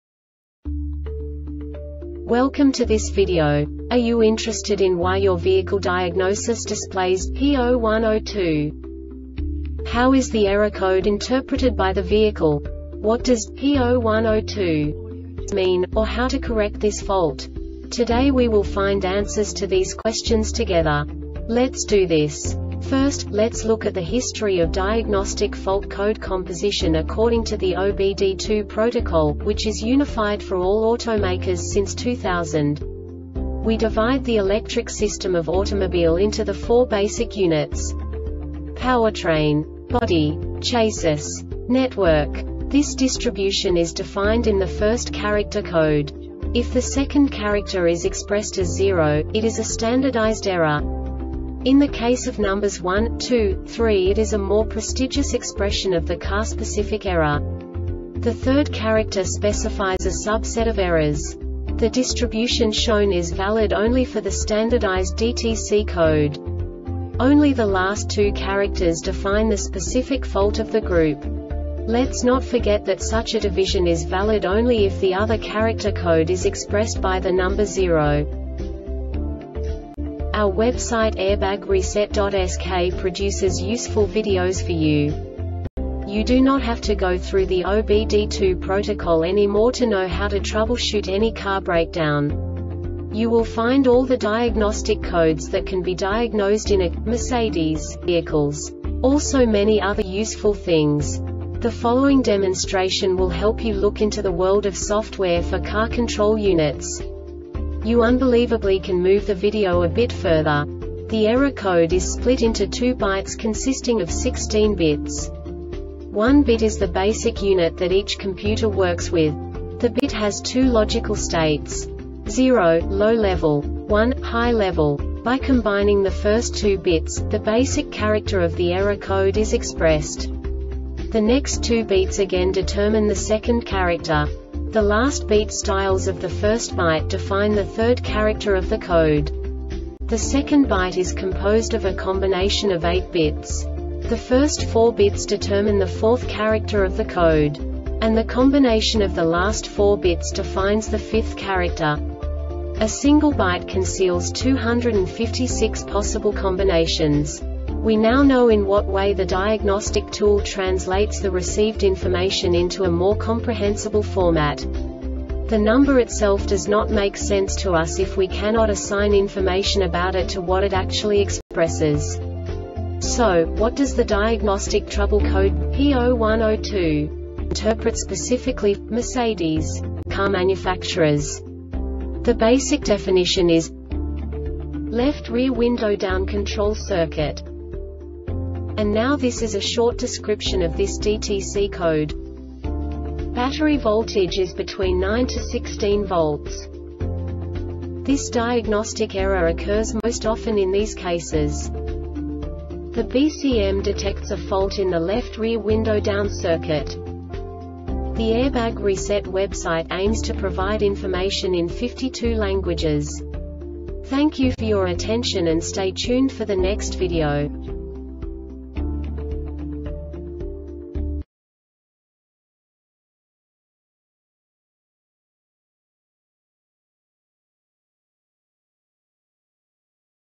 DTC Mercedes-Benz P0102 Short Explanation
Contents:
0:21 Basic DTC analysis according to OBD2 protocol standard.
1:48 Insight into programming
2:58 A diagnostically understandable description of the Diagnostic Trouble Code
3:05 Basic error definition

Make
Mercedes Benz
Code
P0102
Definition
Left Rear Window Down Control Circuit
Description
Battery voltage is between 9-16 volts
Cause
The BCM detects a fault in the left rear window down circuit